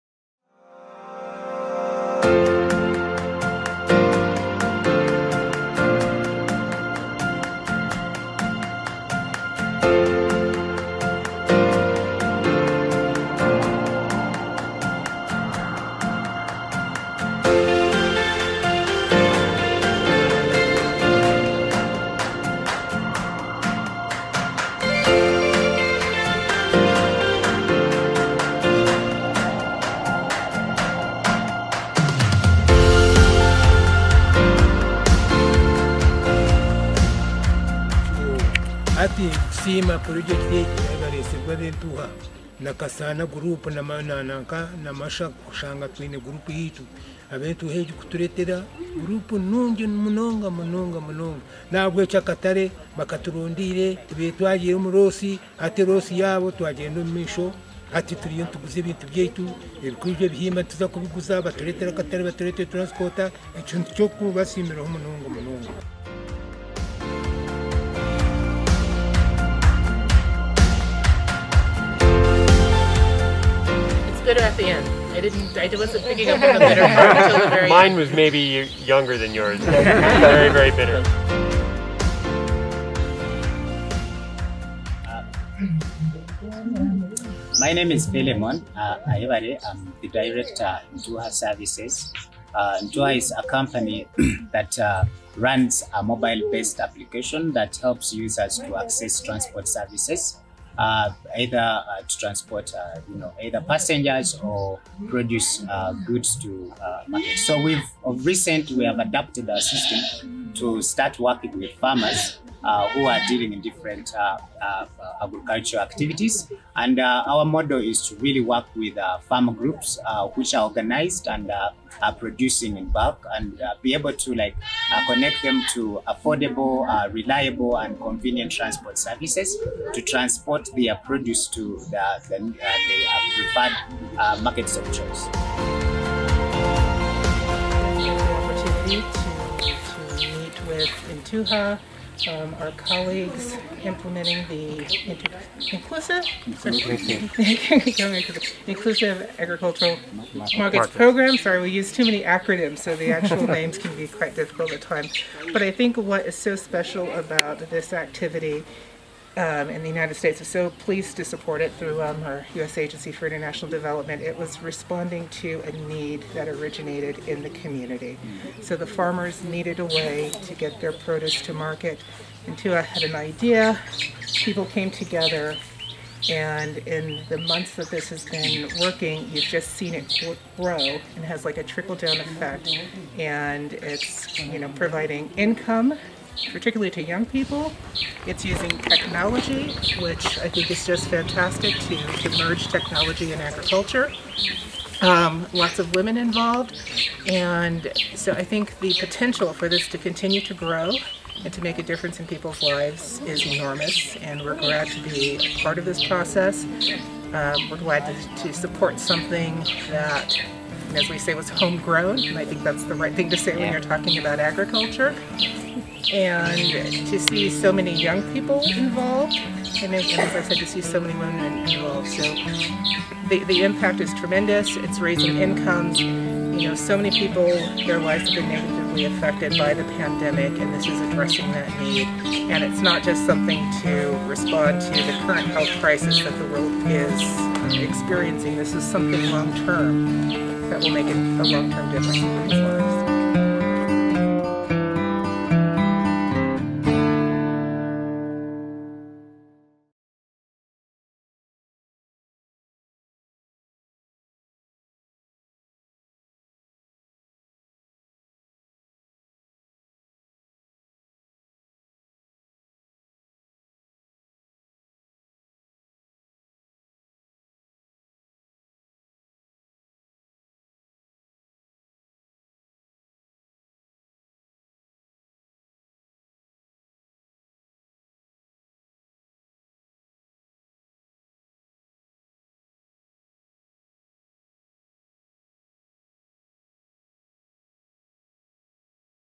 🇺🇸 Ambassador H.E. Natalie E. Brown visit to southwestern Uganda to witness Ntuha activities.
Ntuha is a ride-sharing platform that helps farmers to connect to reliable transportation means via a mobile and USSD application. It’s operating in Southwestern Uganda, and with support from USAID/Feed The Future IAM Activity to boost agriculture in Uganda; Ntuha gives a great experience to farmers. The US Ambassador visited the farmers that use Ntuha platform more so, Conner with a team at Ntuha to understand more about their business.